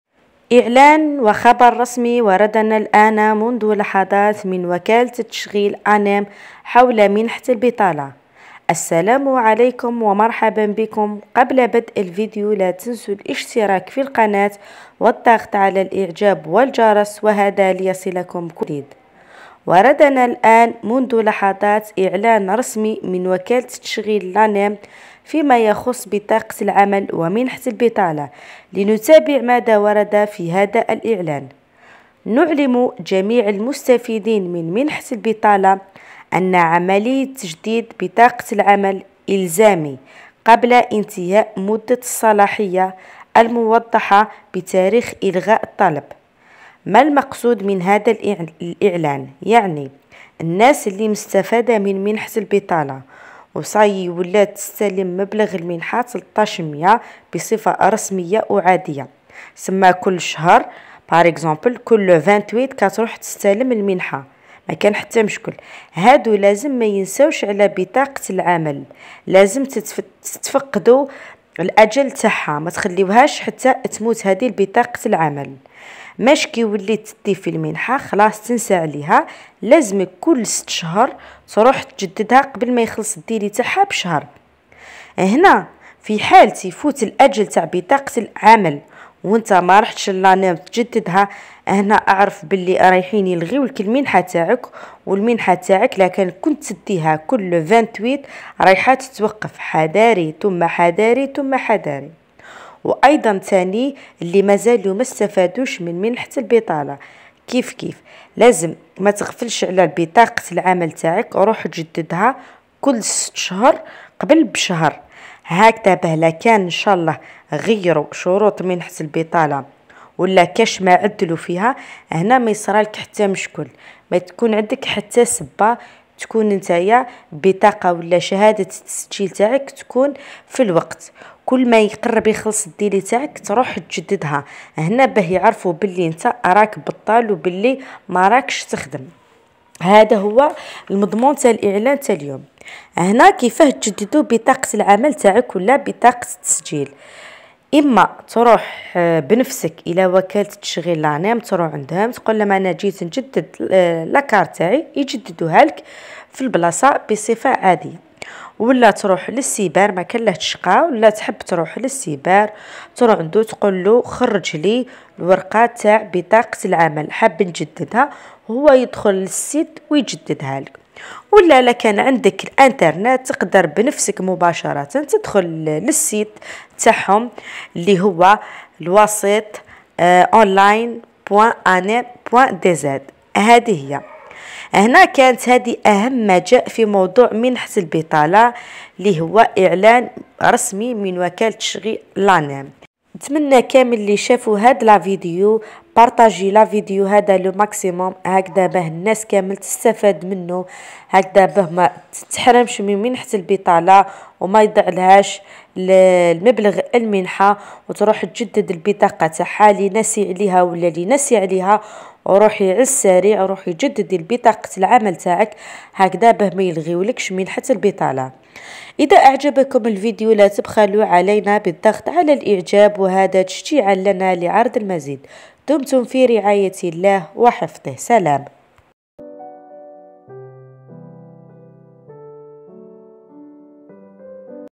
منحة البطالة /خبر عاجل ورسمي وردنا الآن من وكالة التشغيل لانام عن منحة البطالة
منحة البطالة
وكالة_ التشغيل_ لانام